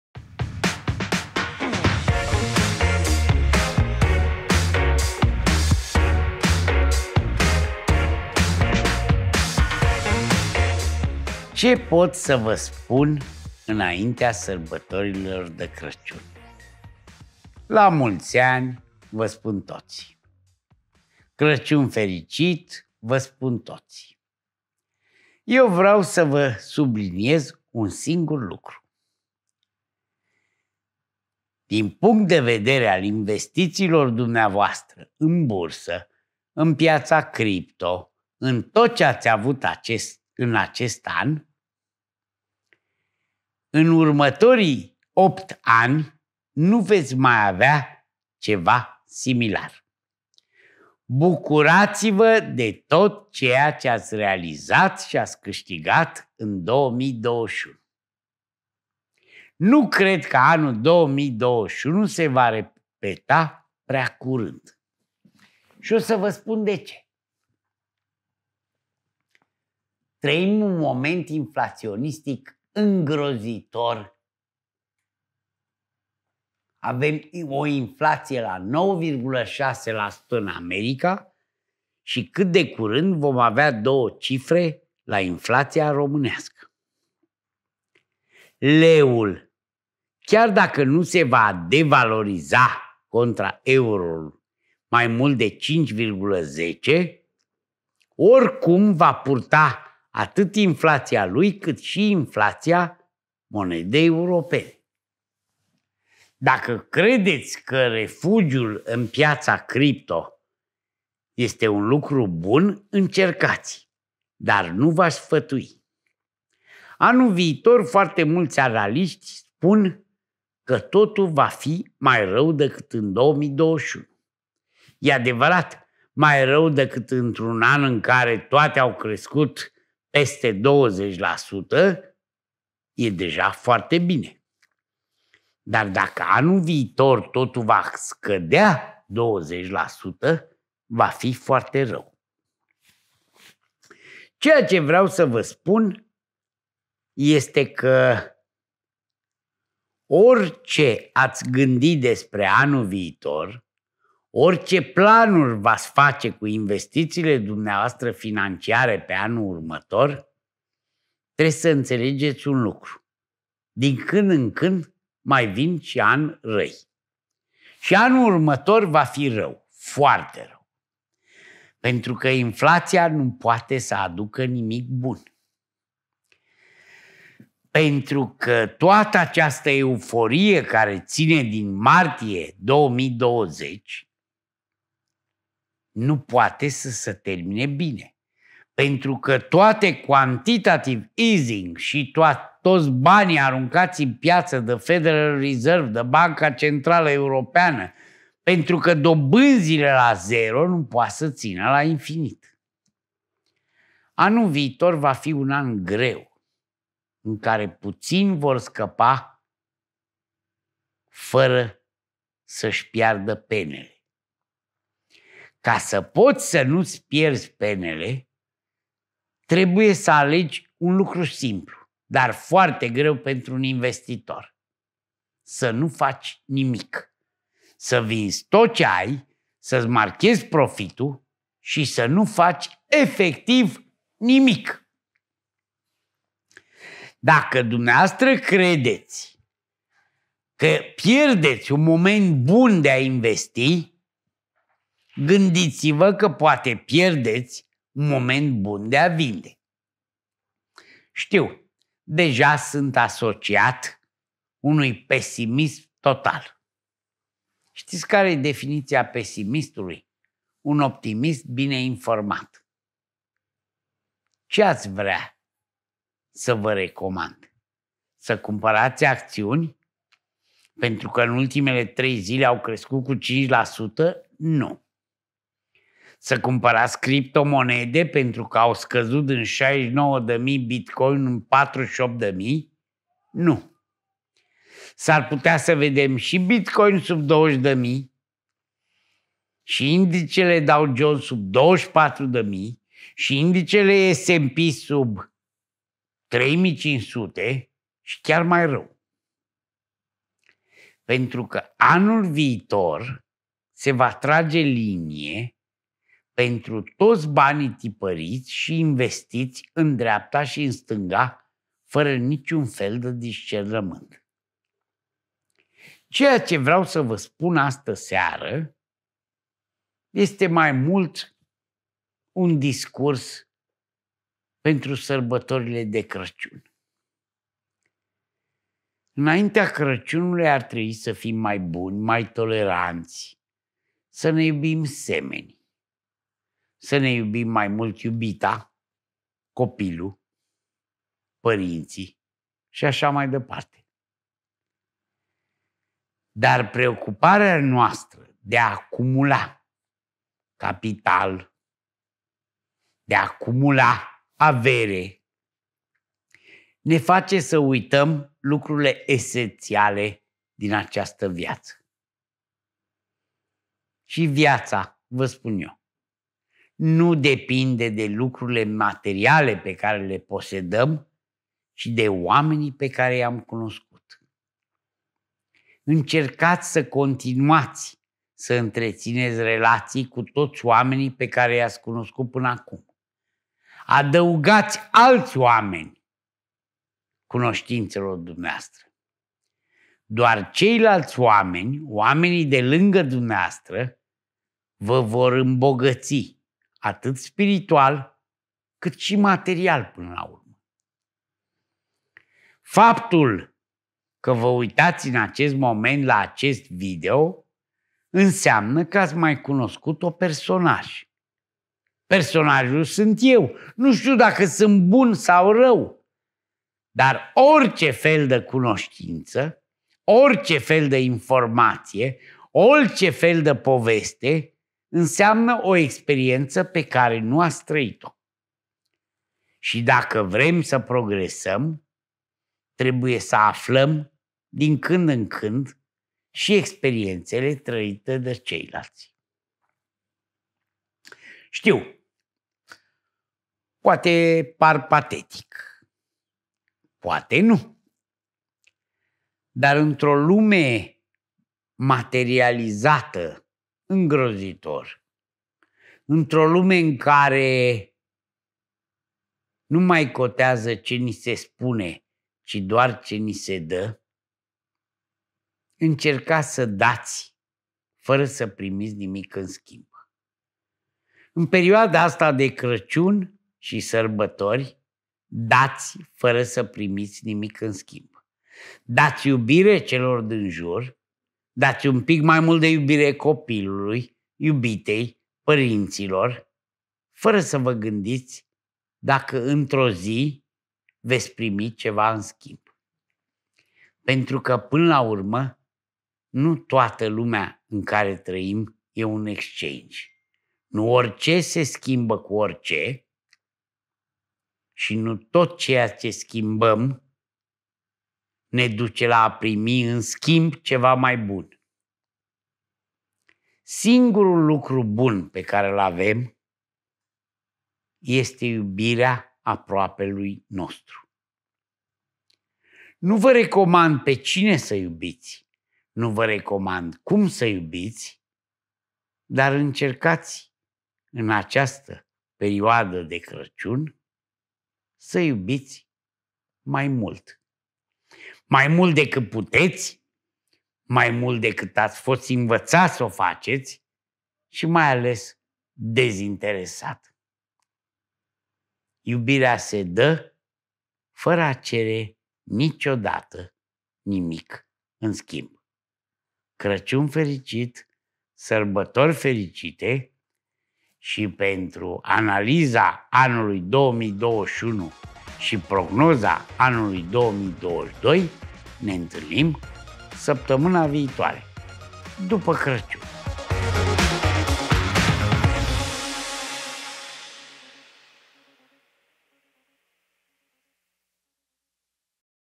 De Crăciun, dăruiește necondiționat! Nu totul e un exchange | Lecțiile lui Cristian Sima
De data aceasta, renunțați la a mai privi tranzacțional. Nu totul e un exchange. Știu, suntem pe un canal despre trading, în mare parte, dar de fapt conexiunile de calitate pe care le avem cu ceilalți ne fac cu adevărat fericiți! Aveți grijă de voi și de cei dragi.
Din punct de vedere al investițiilor în bursă și în crypto, în următorii 8 ani, nu veți mai avea ceva similar.
Nu cred că 2021 se va repeta prea curând. Trăim un moment inflaționist îngrozitor de 9.6% în US și, curând, de două cifre, în România.
Leul, chiar dacă nu se va devaloriza contra euro mai mult de 5.1, oricum va purta atât inflația lui, cât și pe cea a monedei europene.
Dacă credeți că refugiul în piața crypto esre un lucru bun, încercați, dar nu v-aș sfătui! Toate au crescut cu 20% în 2021. Dacă anul viitor totul va scădea cu 20%, va fi foarte rău. Orice planuri v-ați face cu investițiile dvs. fianciare pentru anul viitor, să știți că mai vin și ani răi. Anul viitor va fi rău, foarte rău. Inflația nu poate aduce nimic bun. Euforia care a început din martie 2020, nu poate să țină la infinit și să se termine bine. Anul viitor va fi unul greu, în care puțin vor scăpa fără să își piardă penele. Definiția pesimistului: un optimist bine informat. Anul viitor, se va trage linie pentru toți banii tipăriți fără vreun fel de discernământ.Viața nu depinde de lucrurile materiale, ci de oamenii pe care i-am cunoscut.
Doar oamenii de lângă dvs. vă vor îmbogăți atât spiritual, cât și material
Încercați să dați, fără să primiți nimic în schimb
Nu toată lumea în care trăim e un exchange. Iubirea se dă fără a cere vreodată ceva în schimb.
Mă puteți găsi și aici:
Comandă cele două volume "Marea spovedanie a brokerului fugar" - despre momentul WBS, cât și despre alte subiecte economice și social-culturale.
Cărțile pot fi achiziționate de aici